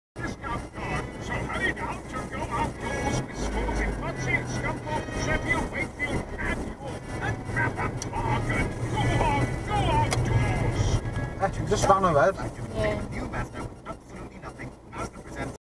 Moron can't read signs, runs the red. VN57 0LK
The sign clearly says, right turning traffic prepare for second signal. But this guy just goes through it